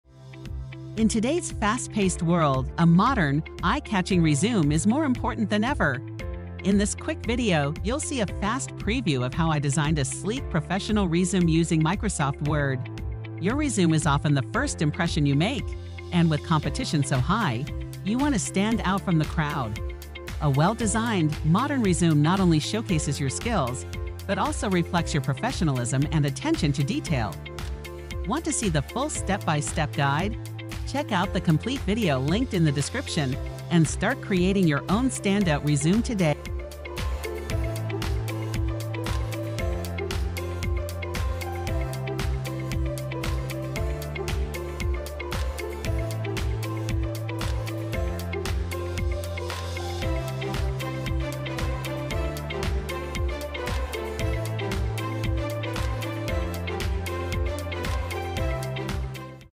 Modern Resume Design in Microsoft Word | Quick Preview
In this quick preview, watch as I create a sleek and modern resume in Microsoft Word, all in under a minute! A modern resume is essential in today’s competitive job market, helping you make a strong first impression. This video is a fast-forwarded version of my full-length tutorial where I guide you step-by-step through the entire resume design process. Want to learn how to create your own standout resume?